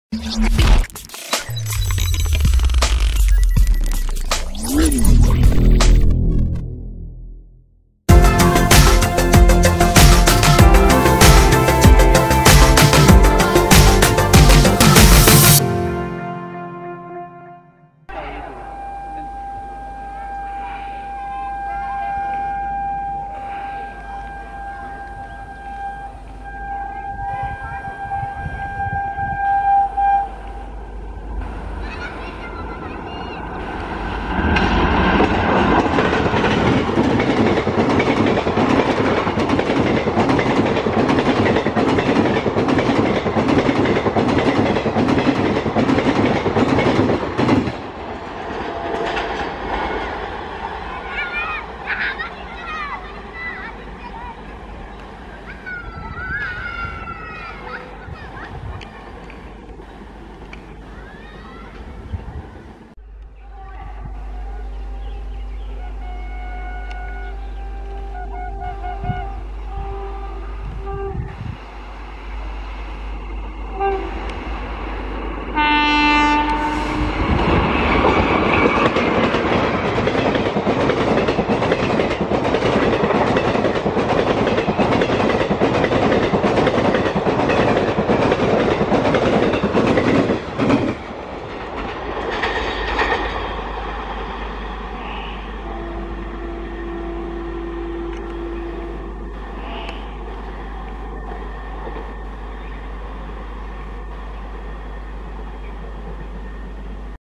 15 AUGUST SPECIAL VIDEO.WAP4 VS WAP5.13016 Jamalpur Kaviguru Exp With Tri Colour Wap4 And Wap5.
Device Use-Canon Sx540Hs.